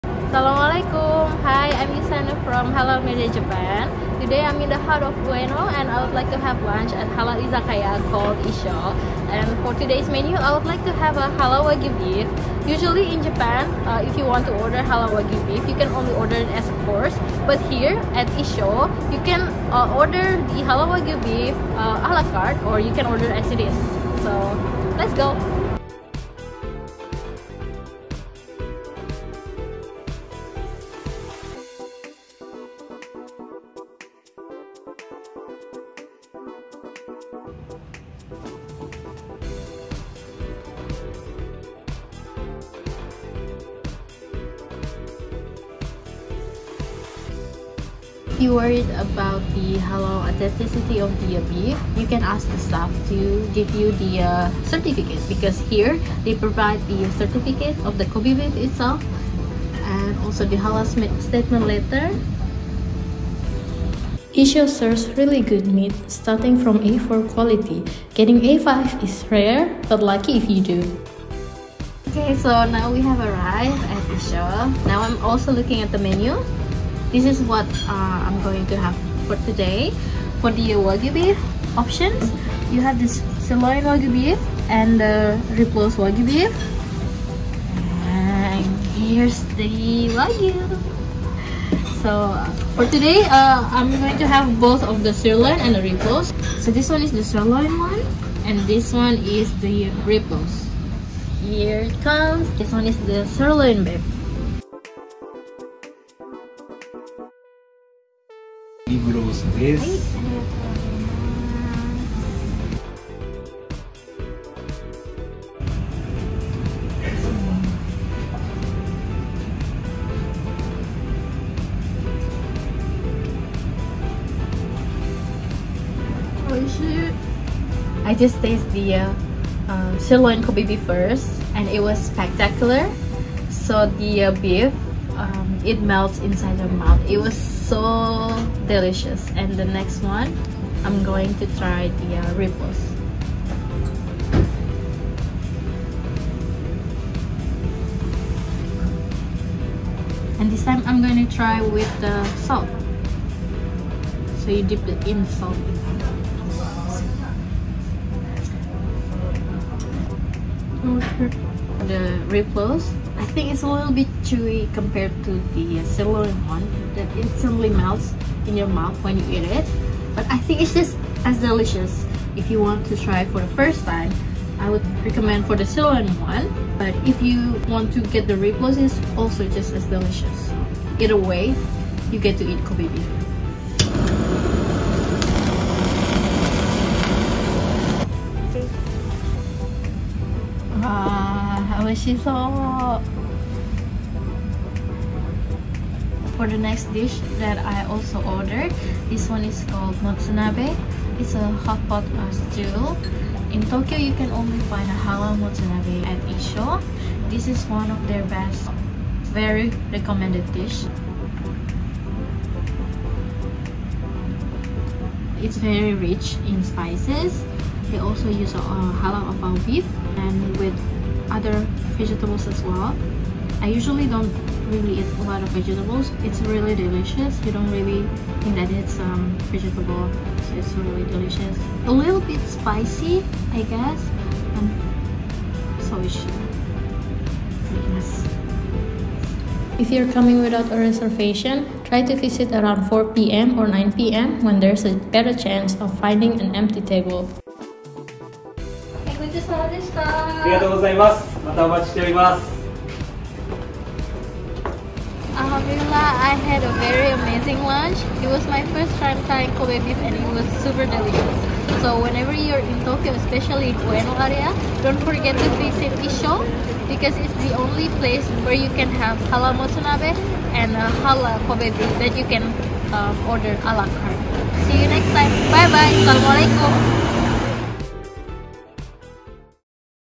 Halal Kobe Beef: Tokyo's Must-Try Expensive Japanese Beef!
Indulge in the epitome of Japanese culinary luxury at this restaurant offering Halal Kobe Beef à la carte!
If you're a connoisseur seeking the finest in Japanese beef, this spot deserves a prime spot on your list.

About Reservation and Walk-in

To ensure an exceptional dining experience, we highly recommend securing a reservation in advance. You can do so by reaching out via phone or sending a direct message to the restaurant's Instagram (link below).

However, if you find yourself without a reservation, consider visiting around 4 pm or 9 pm, as these times often have a higher chance of securing an available table for a spontaneous culinary delight.

About the meat rank

They exclusively provide the highest quality meats, with A5-ranked meat being an exceptionally rare offering. While fortunate visitors may encounter A5 availability during their visit, it's crucial to note that this exquisite grade is extremely scarce and may not be available consistently.

Issho Ueno Instagram

The Japan's Most Expensive Halal Kobe Beef in Tokyo